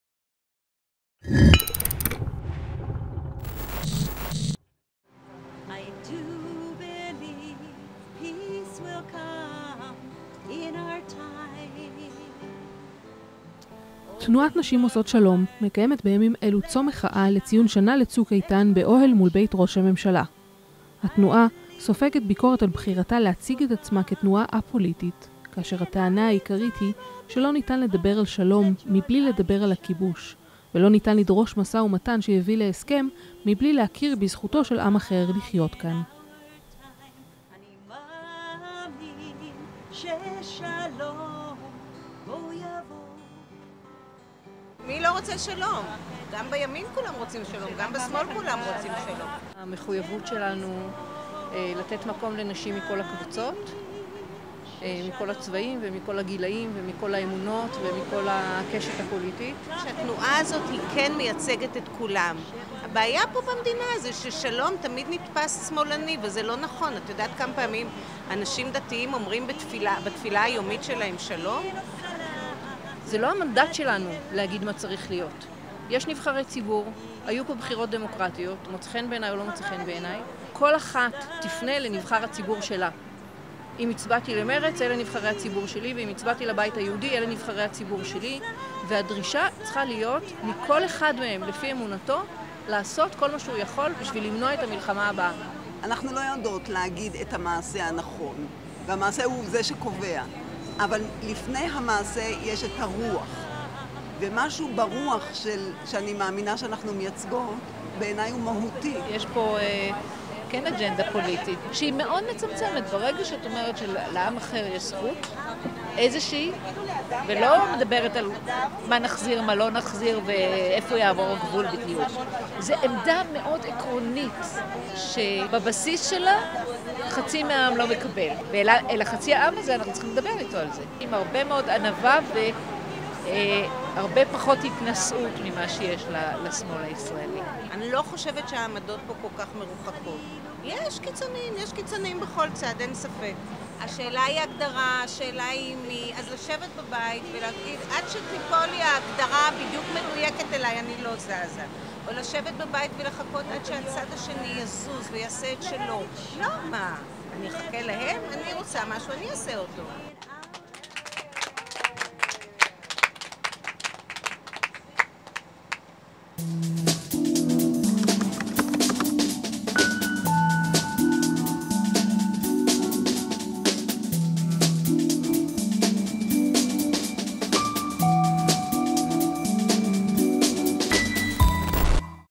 צמ(א)ות לשלום
תנועת "נשים עושות שלום" מקיימת בימים אלו צום מחאה לציון שנה ל"צוק איתן" באוהל מול בית ראש הממשלה.התנועה סופגת ביקורת על בחירתה להציג את עצמה כתנועה א-פוליטית, כאשר הטענה העיקרית היא שלא ניתן לדבר על שלום בלי על הכיבוש ולא ניתן לדרוש משא ומתן שיביא להסכם מבלי להכיר בזכותו של עם אחר לחיות כאן.